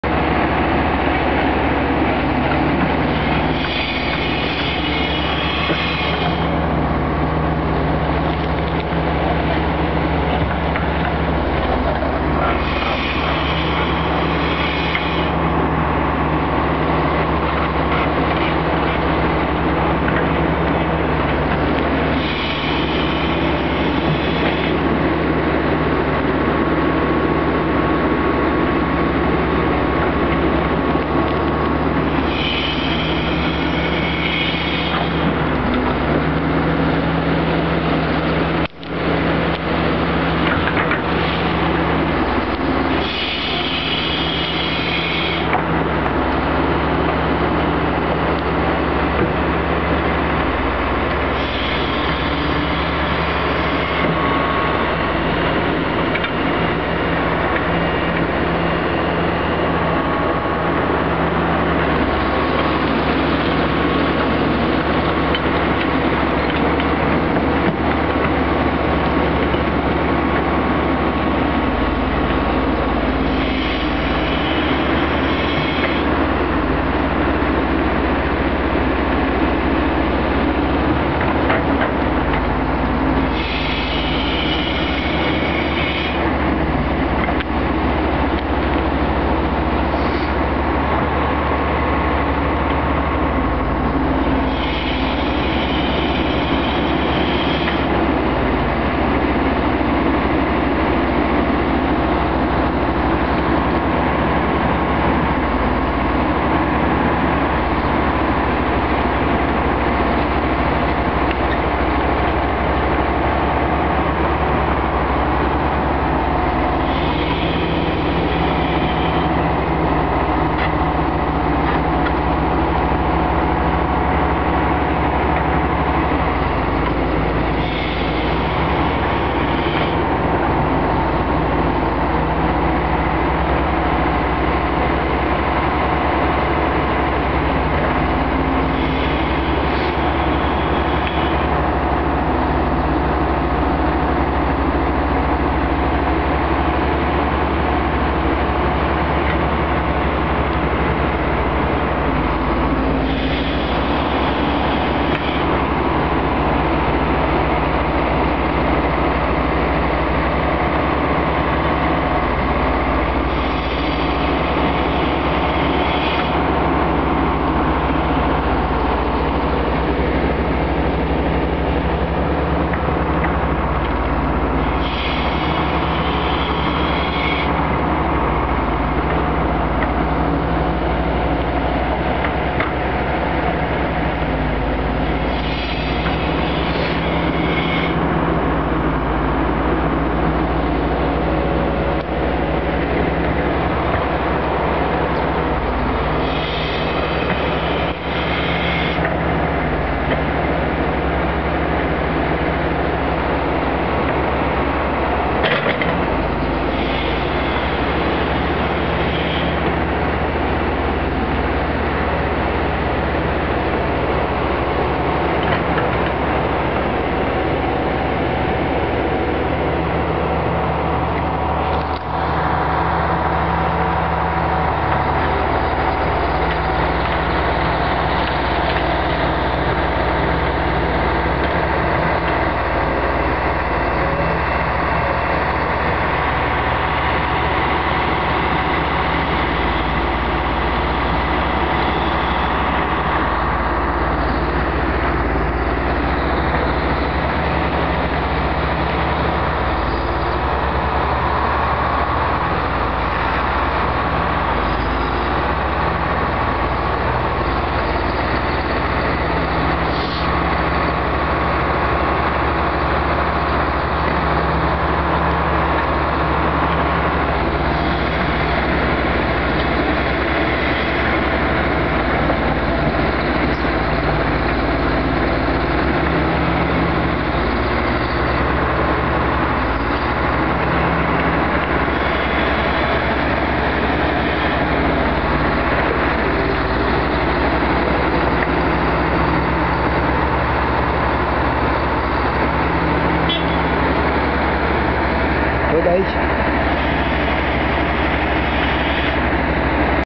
terex atlas excavator
in a old pond making some drainages.